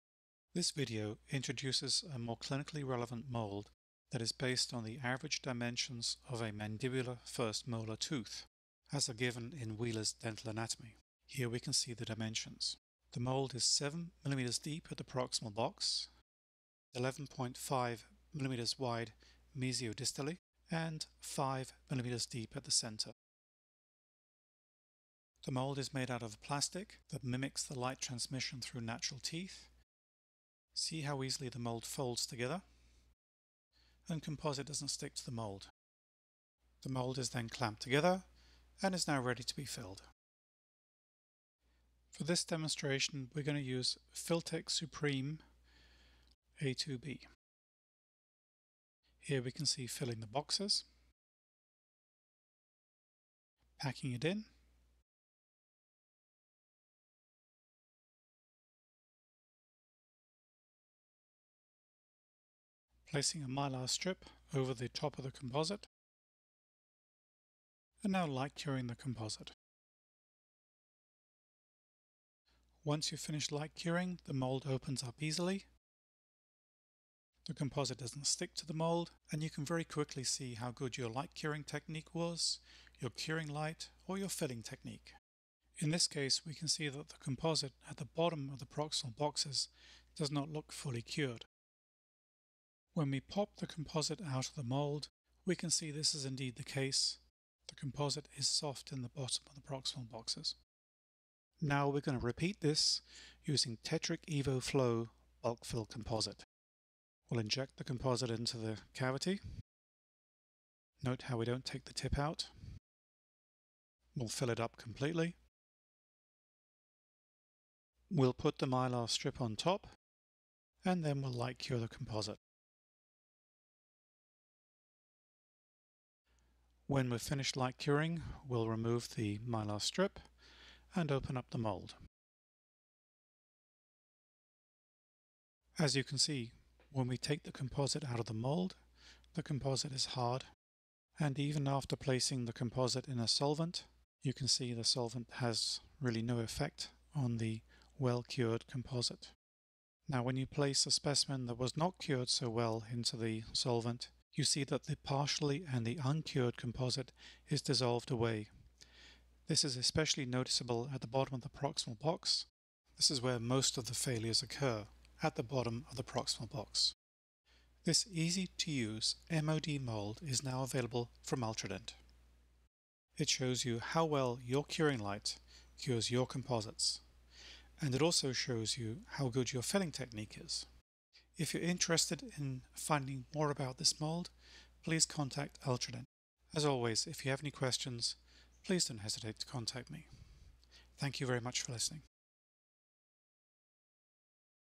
A novel MOD mold to test curing lights, composites, and filling technique
This novel easy-to-use mold shows you how well your curing light cures your composites and also shows you how well you can fill a large cavity with composite.
The use of Filtek Supreme Ultra A2B in the large mold was for demonstration purposes only.
Always follow the manufacturer's instructions. Do not attempt to fill such a large cavity in one increment using conventional, non-bulk fill composites. It does not work.
Please contact Ultradent for more information about the MOD mold.